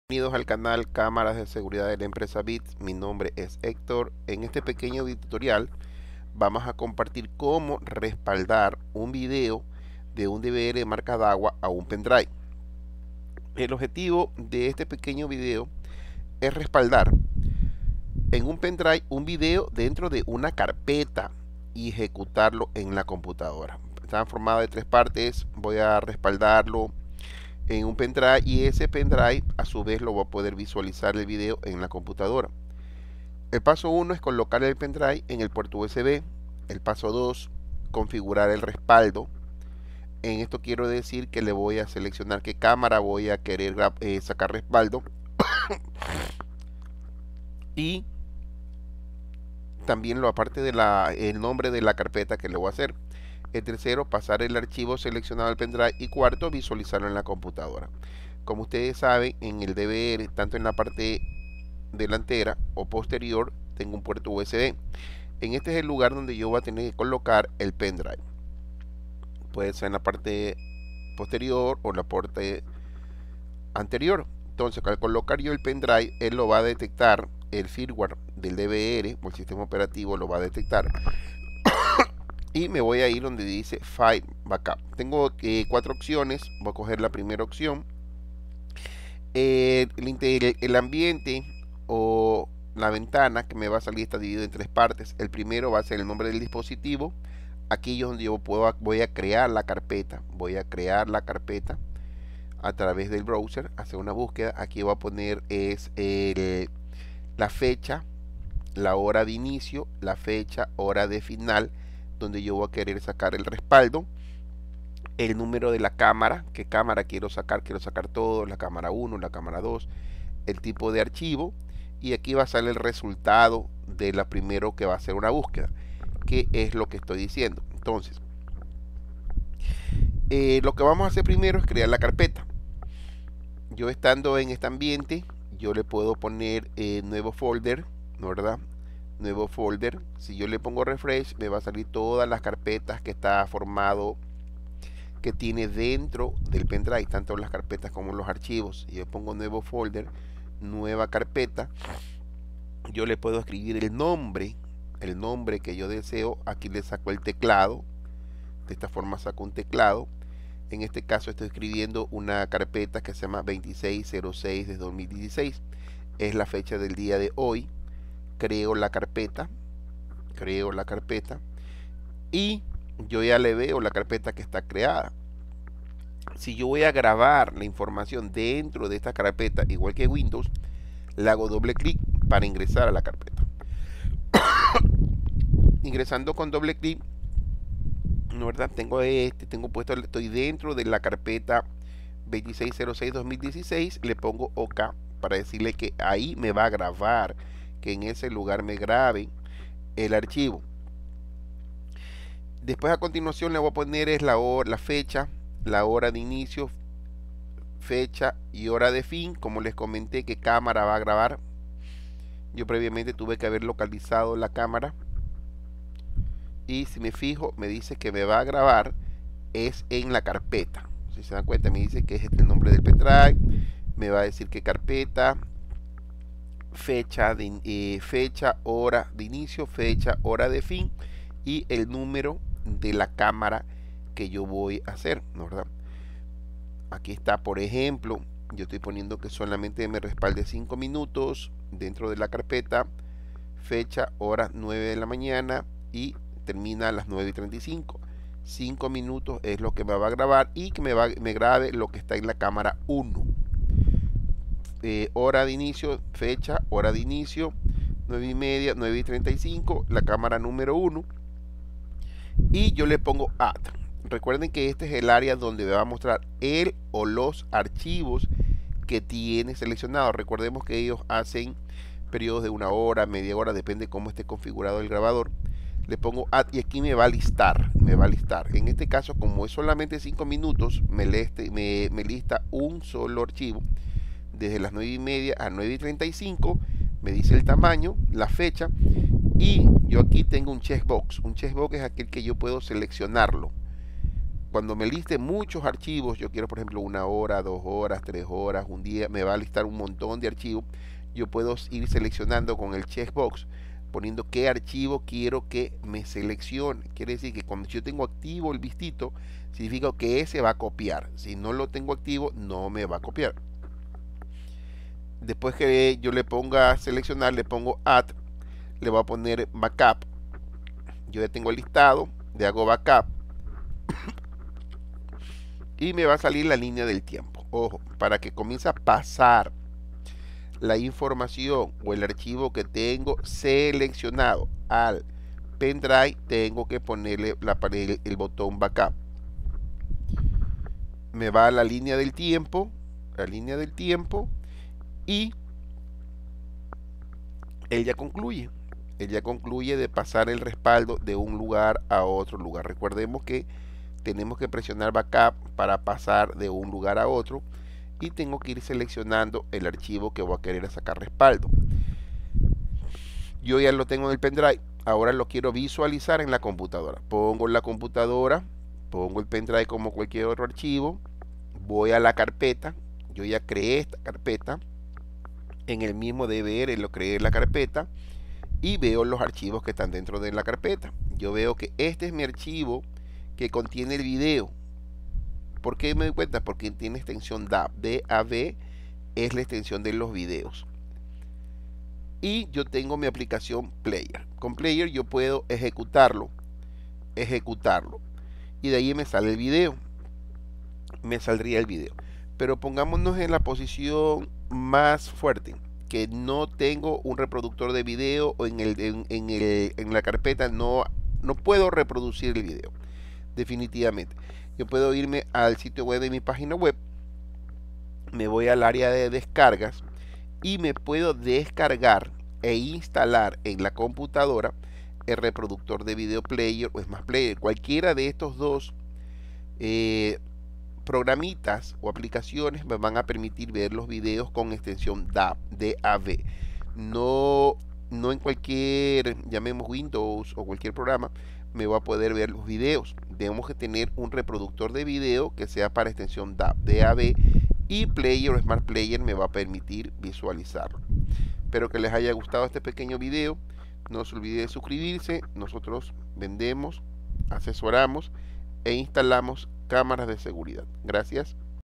Respaldo PenDrive y Visualización PC
Como respaldar uno o varios videos en un pendrive y luego poderlo ver en la computadora.

El grabador es de marca dahua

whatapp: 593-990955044

Guayaquil - Ecuador